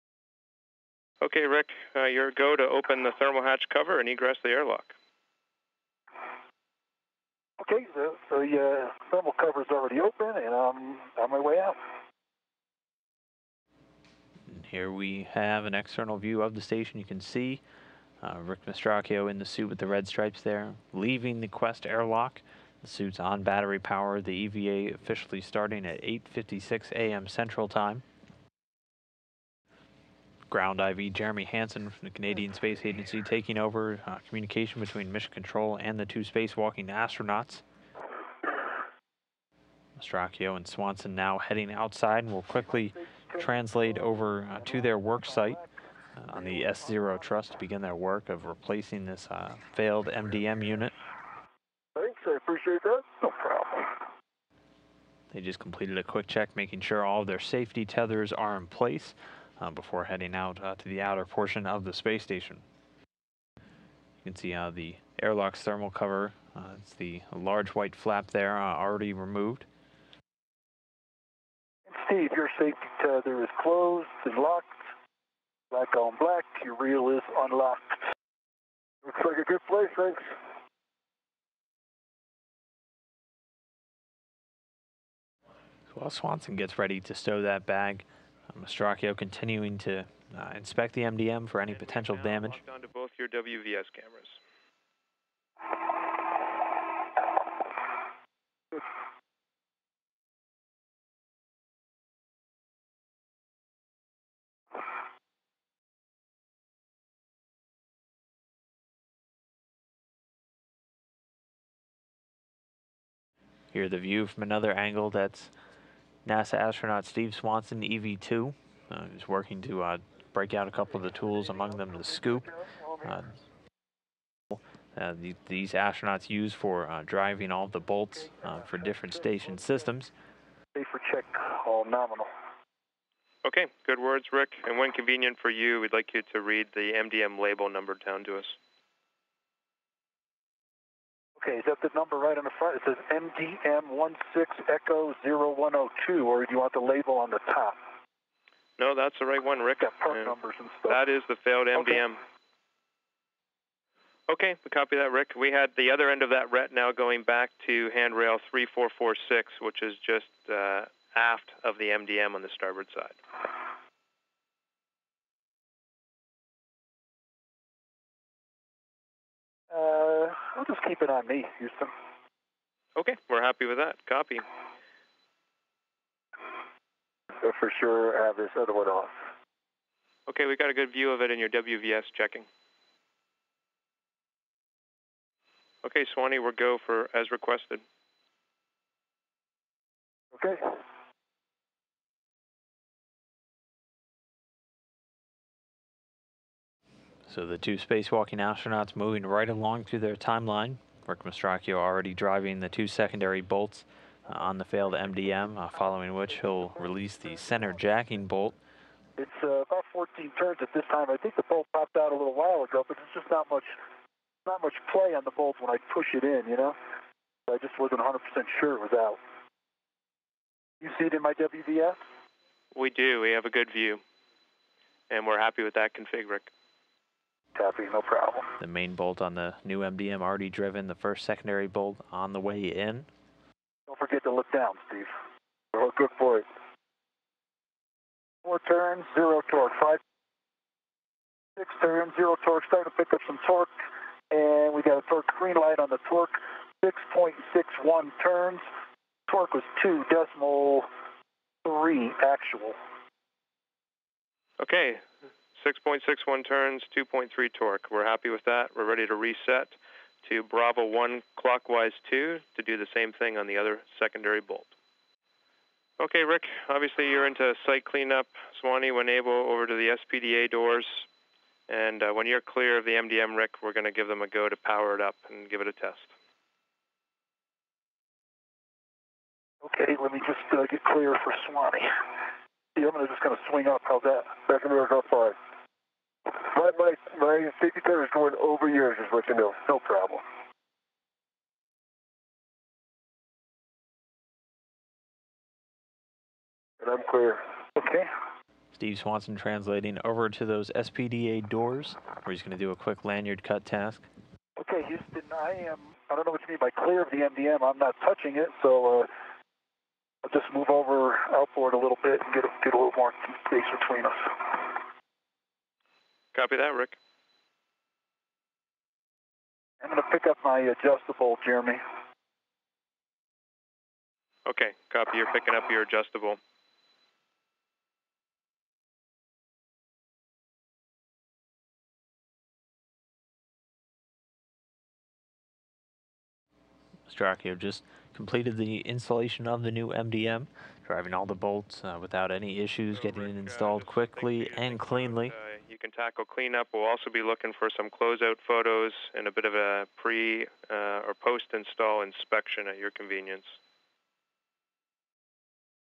Astronauts Replace Relay Box During Spacewalk | NASA Space HD
- astronauts Rick Mastracchio and Steve Swanson conducted a spacewalk on April 23, 2014 to replace a backup computer relay box on the station's S0 (s-zero) truss.

Credit: NASA